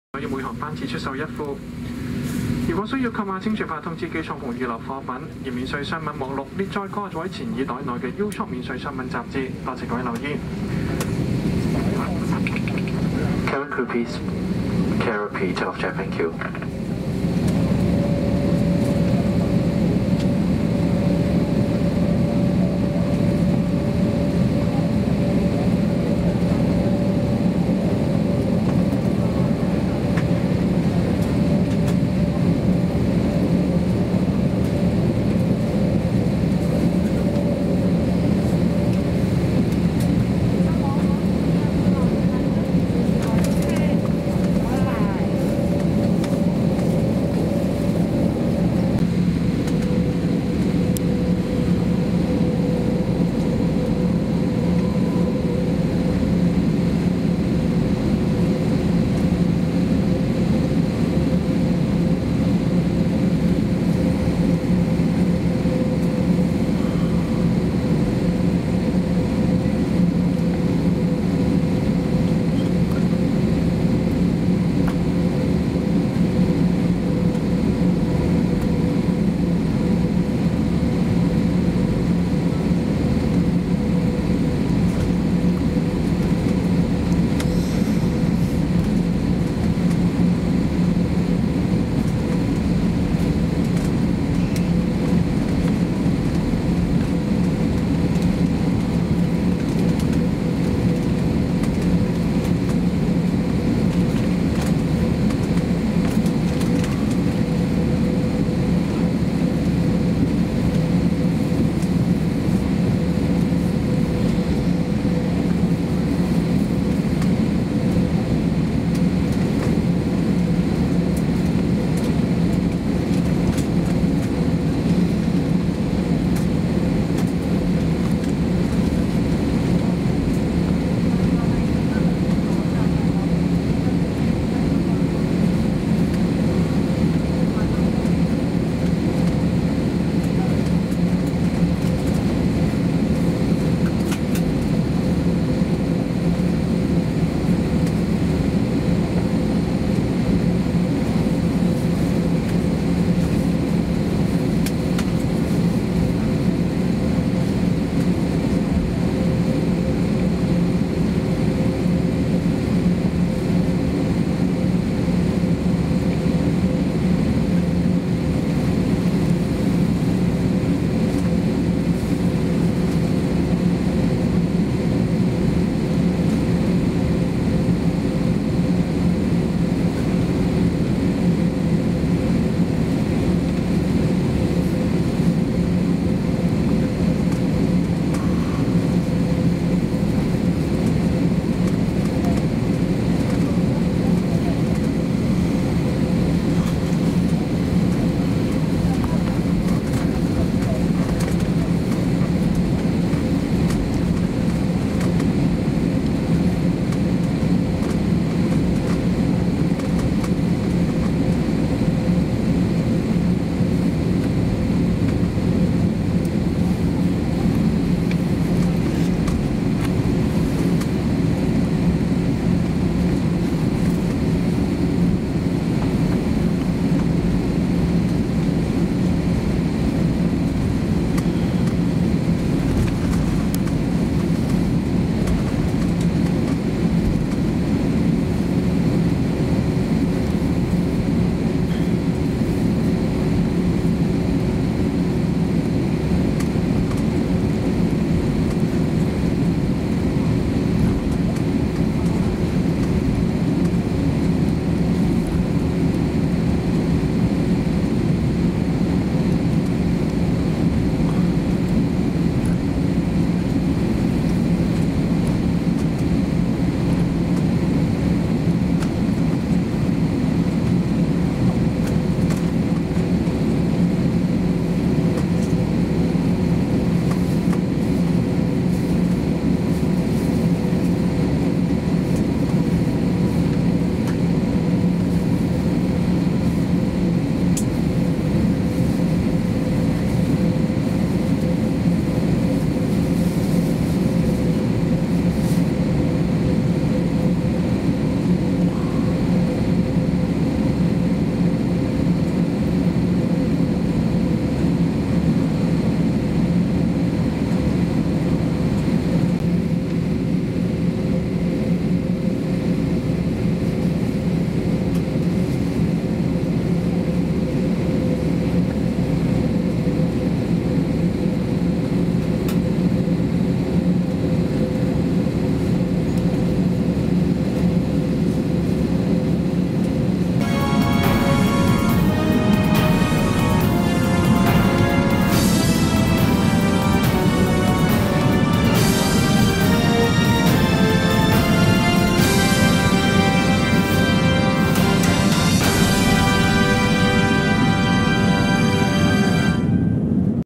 福岡空港タキシング／香港エクスプレス福岡→香港(HKexpress UO601 FUK to HKG)
香港エクスプレスHKexpress UO601(A321-200) 福岡空港→香港国際空港、福岡空港タキシング～RWY34離陸直前までの映像です。

Everyday Update!